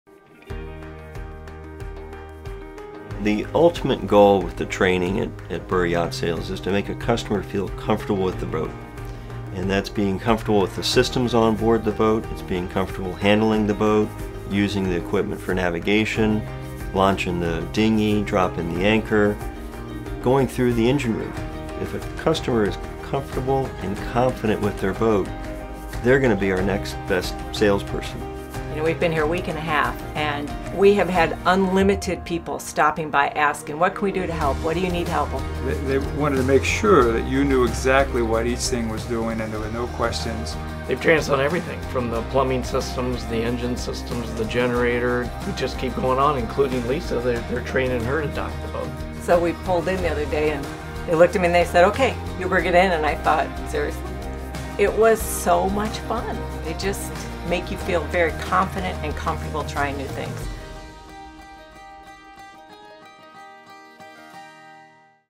This Is How Training Should Be When Buying A Yacht
Fleming builds some of the most technologically advanced yachts in the world that are capable of cruising almost anywhere. When you purchase a new Fleming, our dealer network offers thorough and continuous training of all of the systems of your yacht until you are completely comfortable. If a husband and wife purchase a Fleming, both new owners will receive training on handling and docking until everyone is proficient. This is how your training should be when buying a new boat.

Come experience the different that the Fleming dealer network has to offer with the world's ultimate cruising yacht,